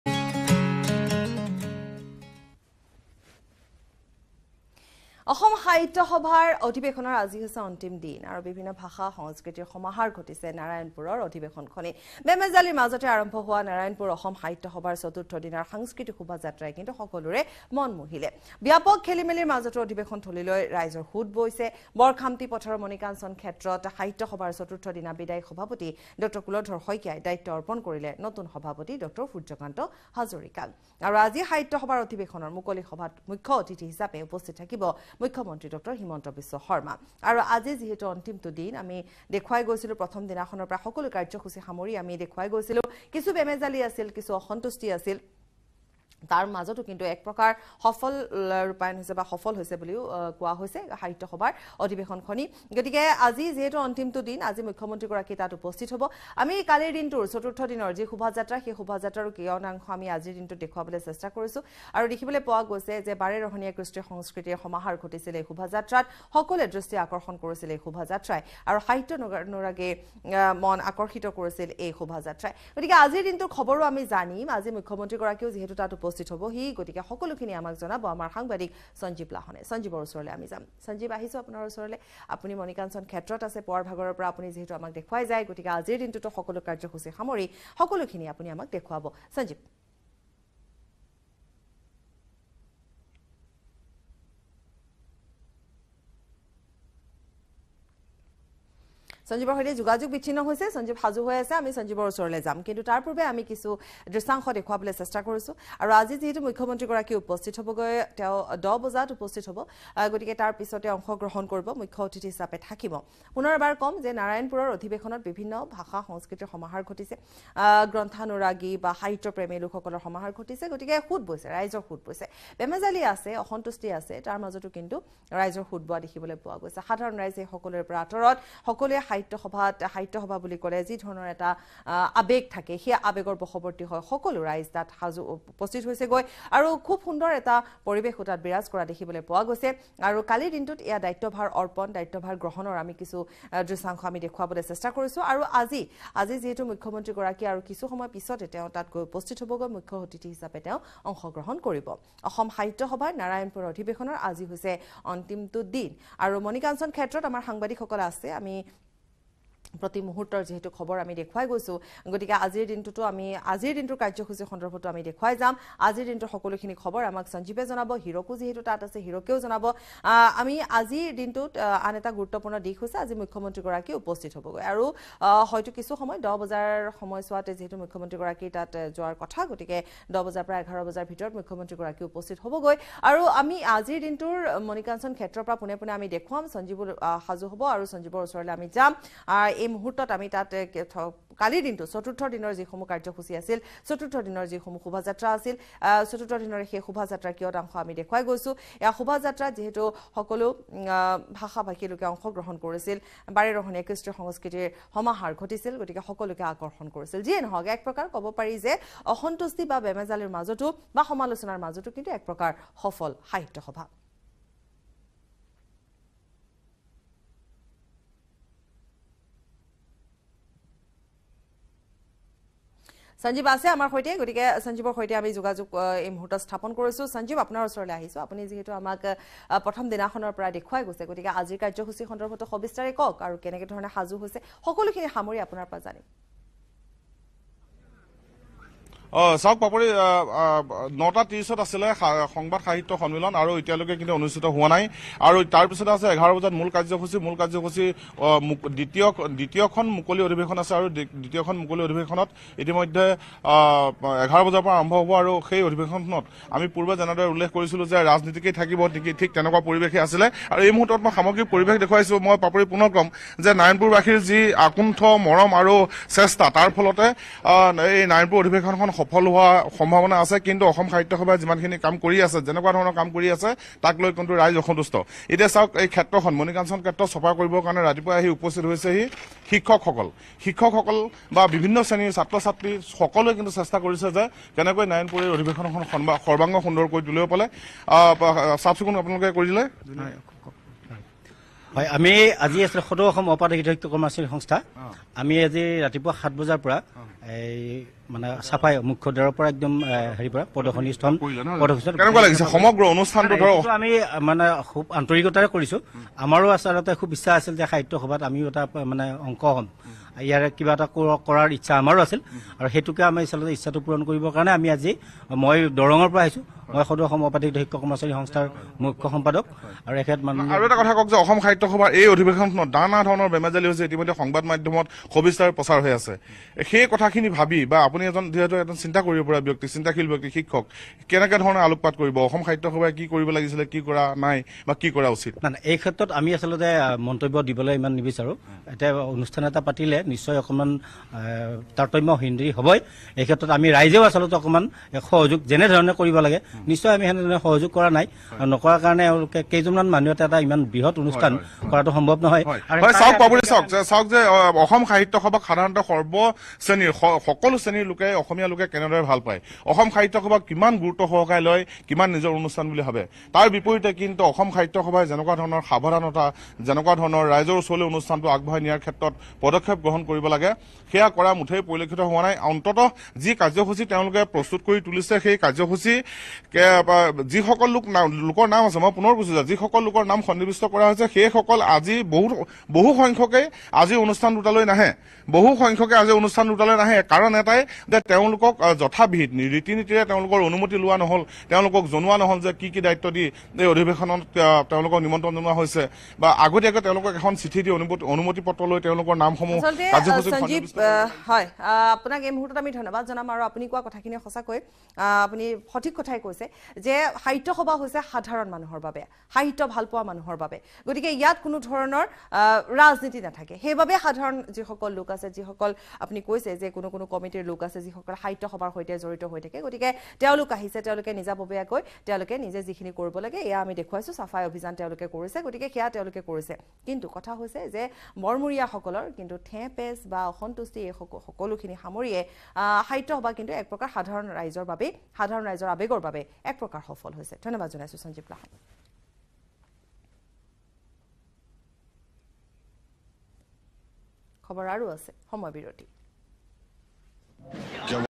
Assam Sahitya Sabha 2023 | অসম সাহিত্য সভাৰ অধিৱেশনৰ আজি অন্তিম দিন | Assamese News
Assam Sahitya Sabha 2023 | বিভিন্ন ভাষা-সাংস্কৃতিৰ সমাহাৰ ঘটিছে নাৰায়নপুৰ অধিৱেশনত